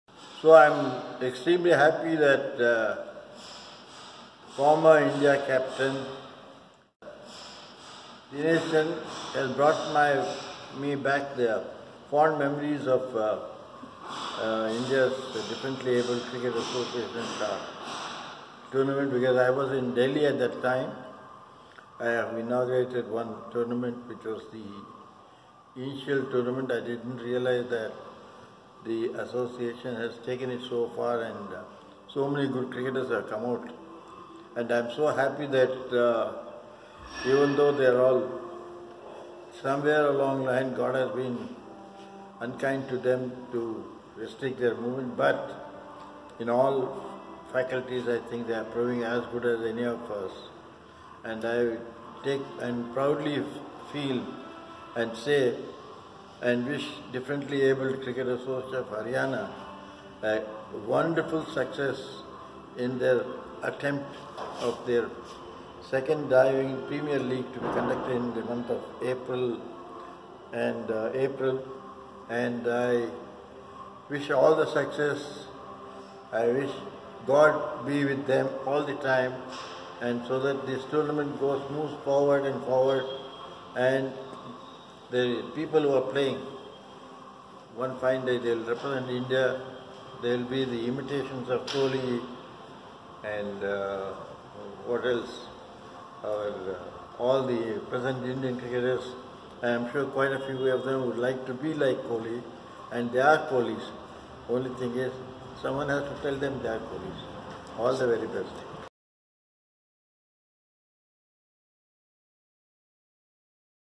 E A S Prasanna - Former India Cricketer gives his best wishes to Divyang Premier League 2021
Former Indian cricketer Mr. E A S Prasanna, like his name very happy about the upcoming Divyang Premier League.